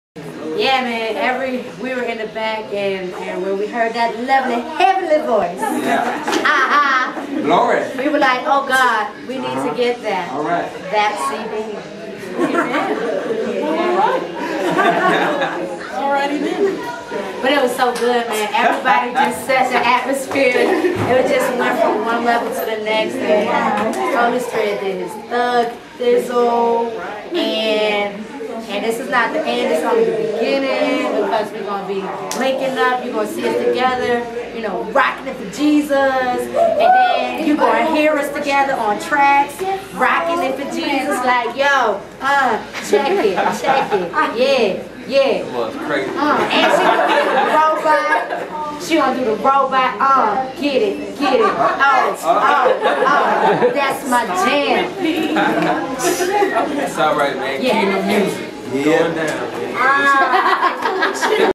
Erica Cumbo and Shawny Wright joking after Concert Together
Shawny Wright of 627 Music and Erica Cumbo of CaJo Records having a good time after concert together.